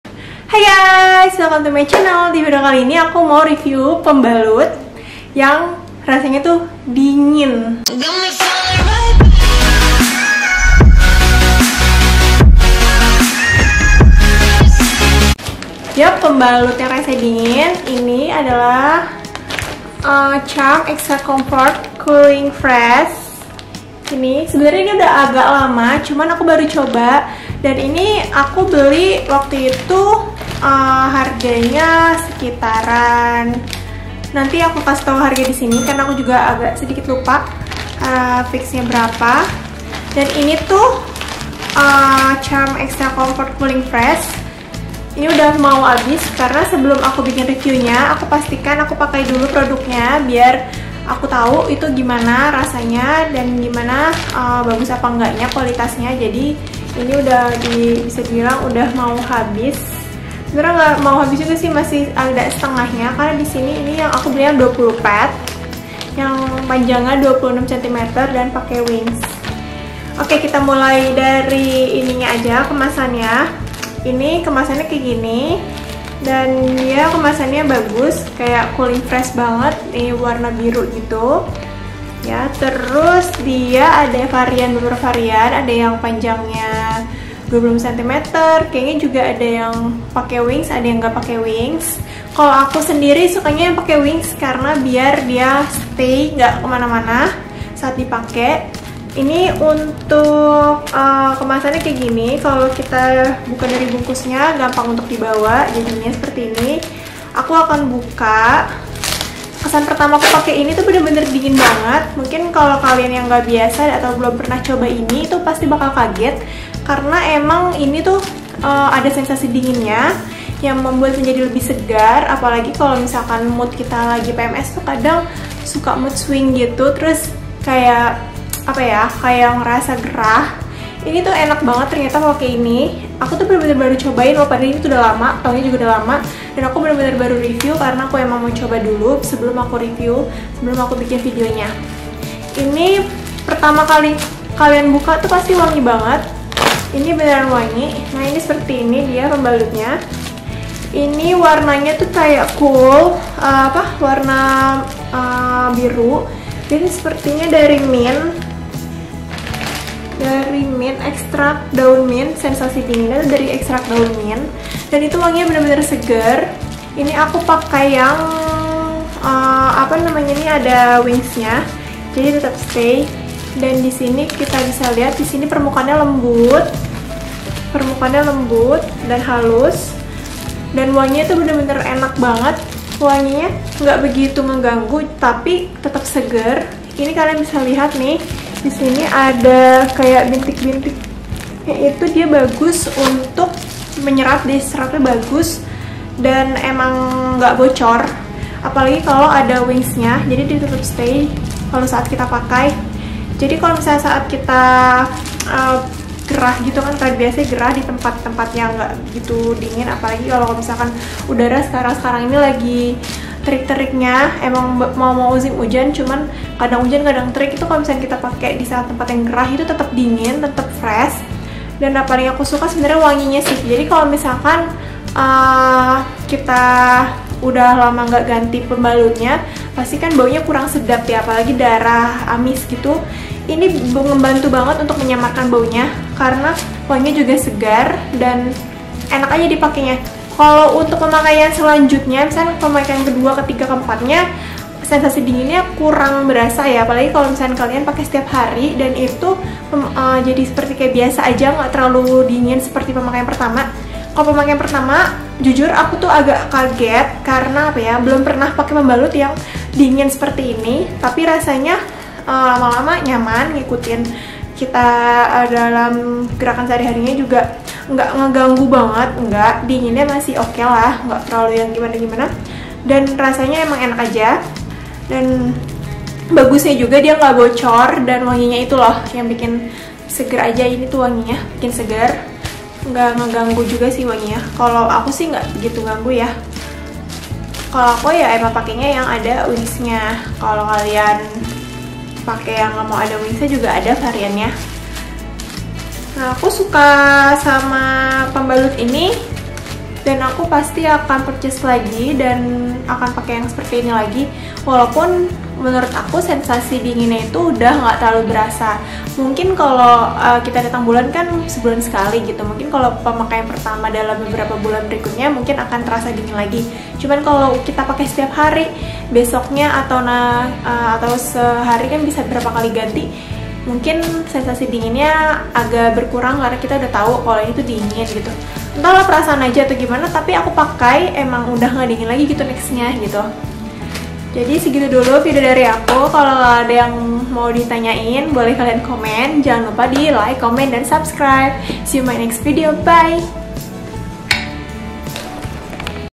REVIEW Charm Extra Comfort Cooling Fresh - Sensasinya dingin banget❄️
Hallo girlsss, di vidio ini aku mau review pembalut yang lagi aku suka karna ada sensasi dinginnya gitu. simak yukk sampe selesai jangan di skip ya vidionya :* happy watching :* xoxo :*